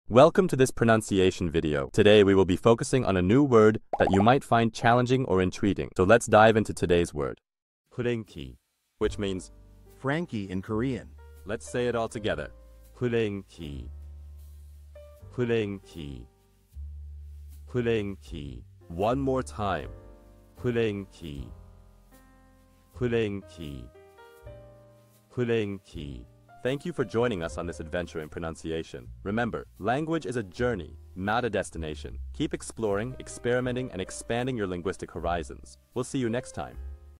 How to pronounce peuraengki | 프랭키 (Frankie in Korean)
Master the Pronunciation of 'peuraengki | 프랭키' - which means : Frankie in Korean 📝 with @PronunciationKorean 🎓🗣️ - [Your Guide to Korean]

Learn to pronounce 'peuraengki' perfectly in Korean with our easy and engaging tutorial by @PronunciationKorean! 📖📽️ In this comprehensive video, we provide you with native speaker examples 🗣️👂 and phonetic breakdowns 🎵 to ensure you get the pronunciation just right.